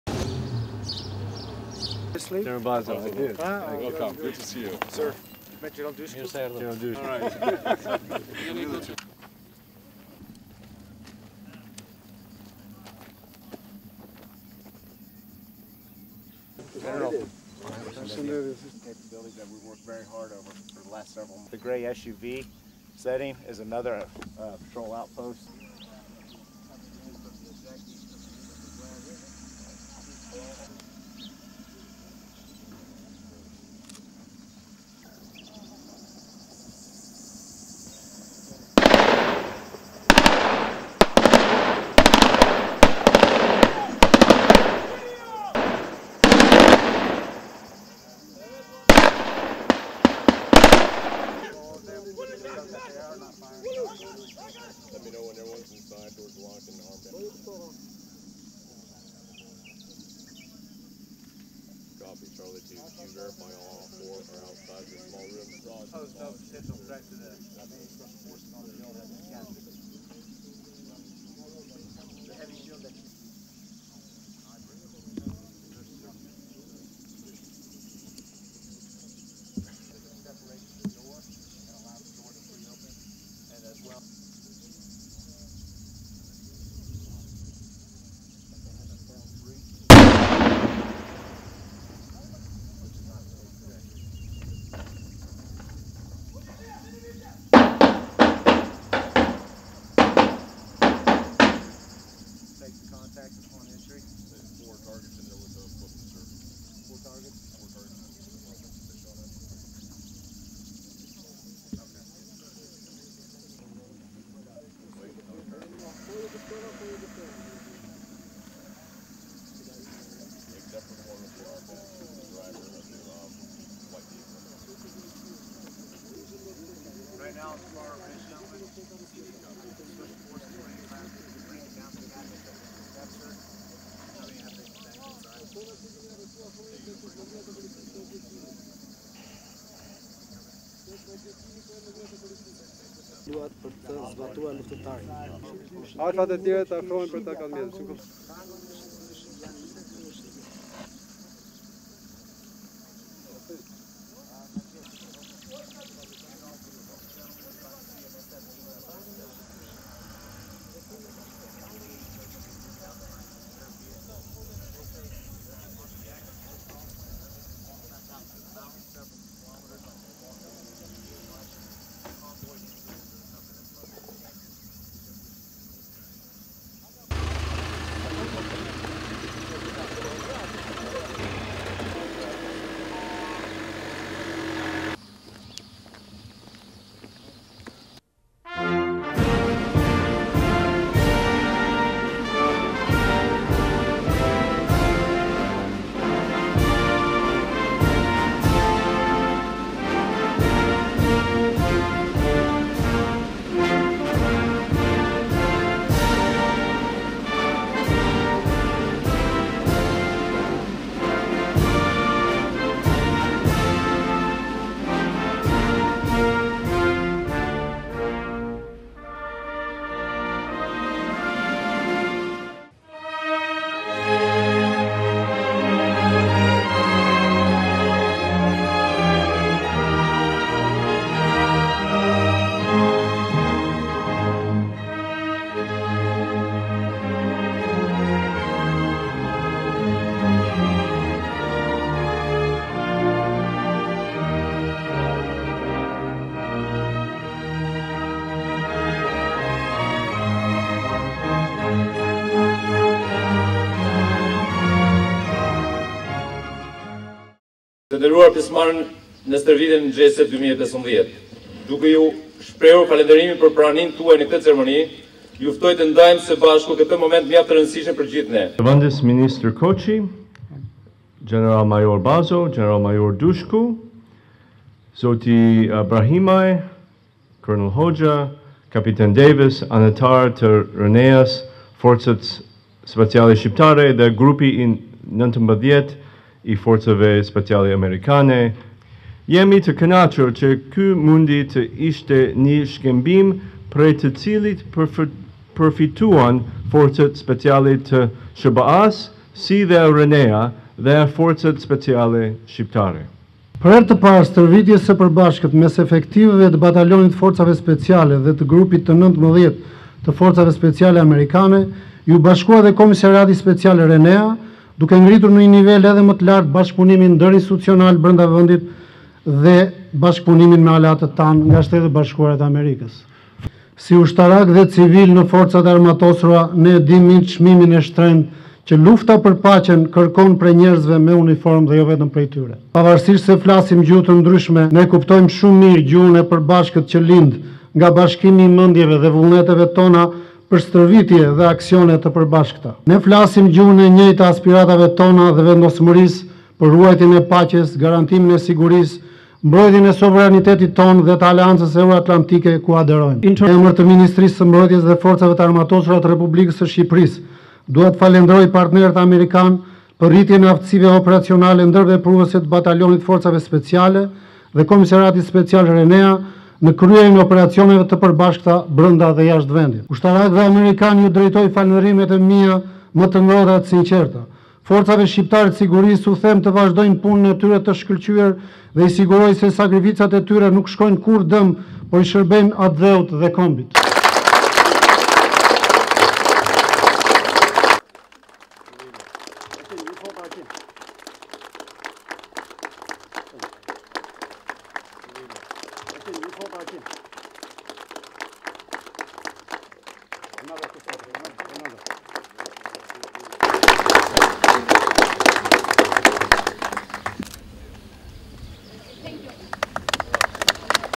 A1 Report - Përfundon stërvitja “JCRT- 2015” Koçi:Sakrificat s'shkojnë kurrë dëm
TIRANË - Ka përfunduar me sukses stërvitja e përbashkët “JCRT- 2015”, e cila u zhvillua në rajonin e Zall-Herrit me pjesëmarrjen e Batalionit të Forcave Speciale, Grupit të XIX të Forcave Specilae Amerikane, si dhe të efektivit të Repartit të Neutralizimit të Elementëve të Armatosur (RENEA) të Policisë së Shtetit. Me këtë rast në mjediset e Batalionit të Mbështetjes së Luftimit në Babru u zhvillua Dita e Vizitorëve të Shquar (DV-Day) dhe ceremonia përmbyllëse e kësaj stërvitje.

Në këtë aktivitet ishin të pranishëm: Zv.ministri i Mbrojtjes, Petro Koçi, shefi i Shtabit të Përgjithshëm i FA-së, gjeneralmajor Jeronim Bazo, gjeneralë, drejtorë dhe ushtarakë të të tjerë të lartë të Ministrisë së Mbrojtjes, Shtabit të Përgjithshëm dhe Forcave të Armatosura etj. Gjithshtu në këtë ceremoni mori pjesë ambasdori SHBA-së në Tiranë, Donald Lu, atasheu ushtarak amerikanë nënkolonel Ralph Shield, si dhe përfaqësues të Zyrës së Bashkëpunimit me SHBA në Ministrinë e Mbrojtjes.

Në përshëndetjen që zv.ministri i Mbrojtjes, Petro Koçi i drejtoi trupave pjesëmarrëse në këtë stërvitje, ndër të tjera, u shpreh se, për herë të parë, stërvitjes së përbashkët mes efektivëve të Batalionit të Forcave Speciale dhe Grupit të XIX-të të Forcave Speciale Amerikane iu bashkua dhe Komisariati Special RENEA, duke ngritur në një nivel edhe më të lartë bashkëpunimin ndërinstitucional mes strukturave të sigurisë brenda vendit, si dhe bashkëpunimin me aleatët tanë nga SHBA.

“Ne e dimë mirë çmimin e shtrenjtë që lufta për paqen kërkon prej njerëzve me uniformë dhe jo vetëm. Pavarësisht se flasim gjuhë të ndryshme, ne e kuptojmë shumë mirë gjuhën e përbashkët që lind nga bashkimi i mendjeve dhe vullneteve tona për stërvitje dhe aksione të përbashkëta. Ne flasim gjuhën e njëjtë të aspiratave tona dhe vendosmërisë për ruajtjen e paqes, garantimin e sigurisë, mbrojtjen e sovranitetit tonë dhe të Aleancës Euroatlantike ku aderojmë”, - tha Koçi.

Në përfundim të fjalës së tij zv.ministri i Mbrojtjes, Petro Koçi, në emër të Ministrisë së Mbrojtjes dhe Forcave të Armatosura të Republikës së Shqipërisë, falënderoi partnerët amerikanë për rritjen e aftësive operacionale ndërvepruese të Batalionit të Forcave Speciale dhe  Komisariatit Special “RENEA” në kryerjen e operacioneve të përbashkëta brënda dhe jashtë vëndit. Një falënderim të veçantë ai i drejtoi ushtarakëve amerikanë dhe forcave shqiptare të sigurisë që morën pjesë në këtë stërvitje, duke theksuar që të vazhdojnë punën e tyre të shkëlqyer, sepse sakrificat nuk shkojnë kurrë dëm, por i shërbejnë atdheut dhe kombit. Gjithashtu trupat pjesmarrëse në stërvitjen e përbashkët “JCRT-2015” dhe të gjithë të pranishmit e tjerë i përshëndeti edhe ambasdori amerikanë në Tiranë, Donald Lu.

Zv.ministri i Mbrojtjes, Petro Koçi, shefi i Shtabit të Përgjithshëm i FA-së, gjeneralmajor Jeronim Bazo, ambasdori SHBA-së në Tiranë, Donald Lu dhe të pranishmit e tjerë në ceremoninë e përfundimit të stërvitjes së përbashkët “JCRT- 2015” ndoqën nga afër një demonstrim nga trupat pjesëmarrëse. Në këtë stërvitje u angazhuan mbi 50 efektivë të Batalionit të Forcave Speciale, të Forcave Speciale Amerikane dhe forcave RENEA.